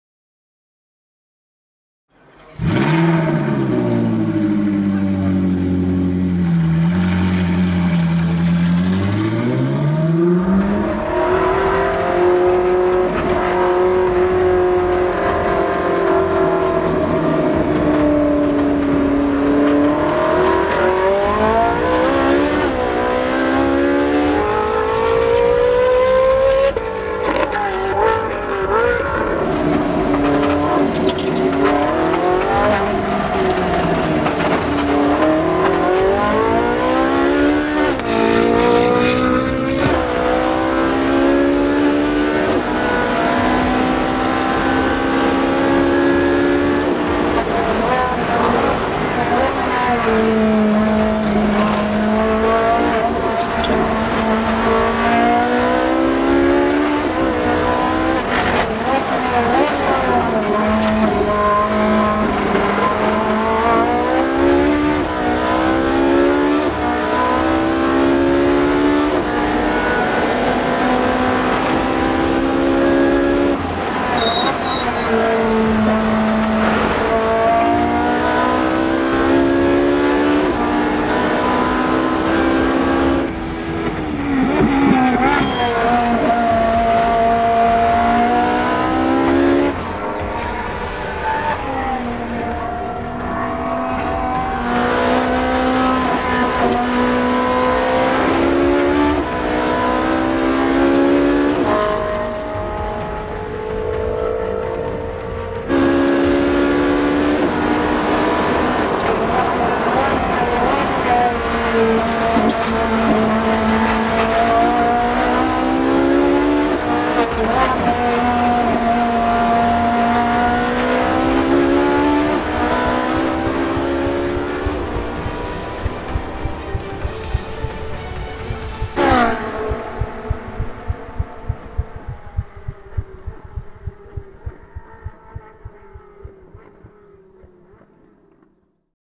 Ferrari 458 Challenge Evo - Hot Lap - Velocita
Full lap on Autodrom Most in Ferrari 458 Challenge Evo. Ferrari race team take us on track. it was hell ride to heaven.

Special thanks to Ivo Horn from Scuderia Ferrari and driver Dan Želinský
Our team Velocita: Jiří Linhart, Vašek Sadílek, Vašek Uhlíř, Michal Jindrák